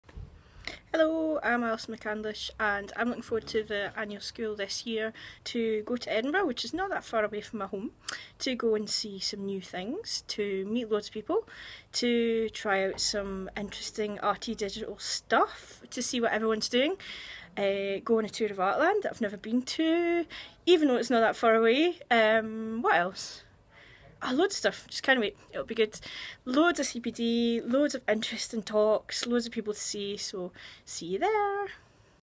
Why I'm looking forward to this years annual IHBC summer school
Some reasons why I am looking forward to this years annual Institute of Historic Building Conservation summer school.

A short video diary/ vlog for the run up to the school which takes place in Edinburgh from 5 - 7 June on the theme "The Art of Conservation".

This was made on my phone, and uploaded via YouTube Capture.